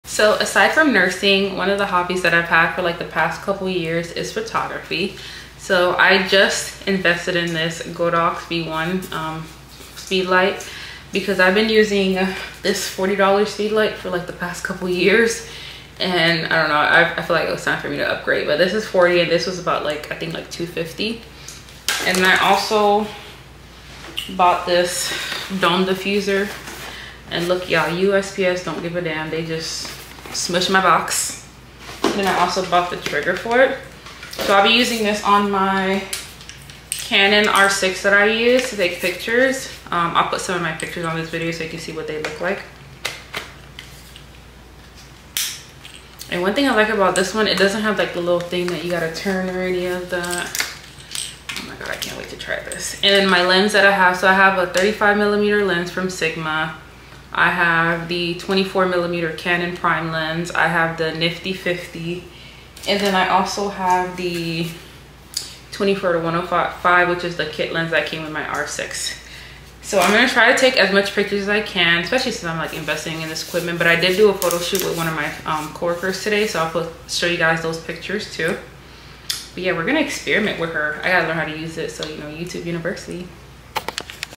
Purchased the Godox V1 Speedlight
I spontaneously purchased the Good V1 speed light. I can't wait to learn how to use it and test it out.